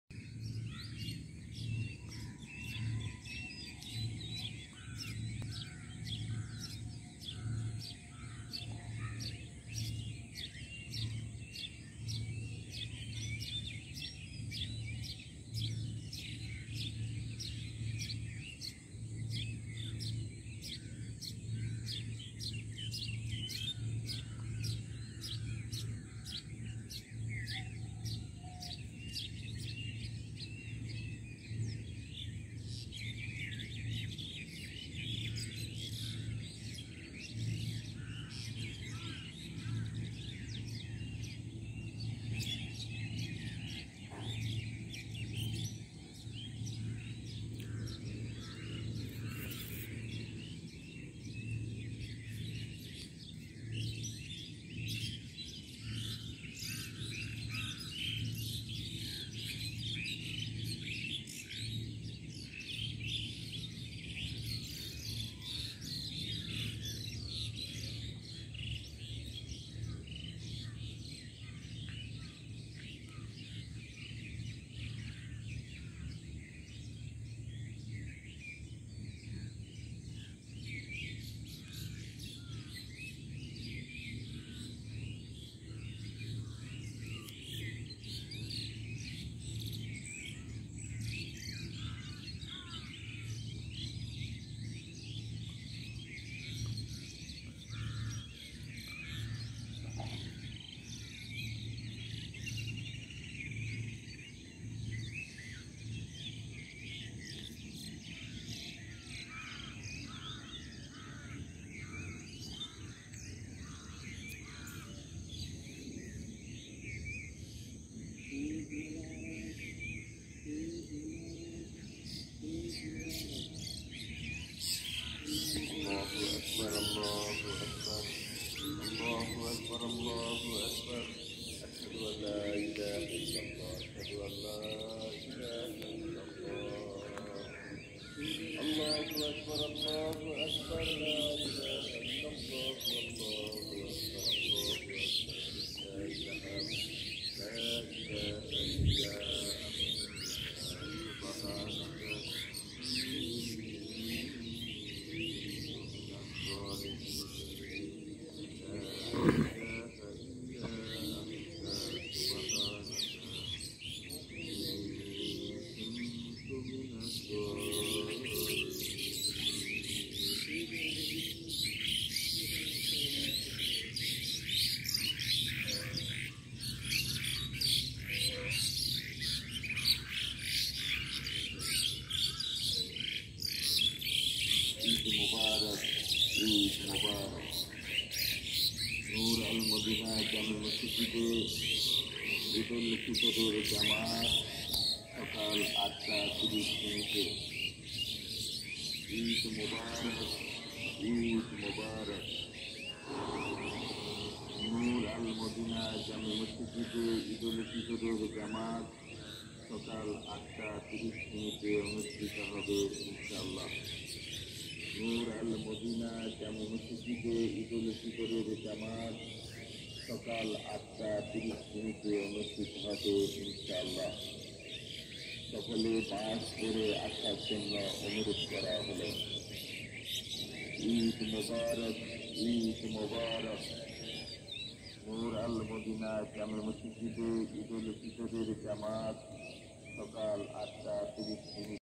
ঈদের সকালের ভিডিও
ঈদের সকালের ভিডিও সাথে পাখির ডাক ও মনোরম পরিবেশ।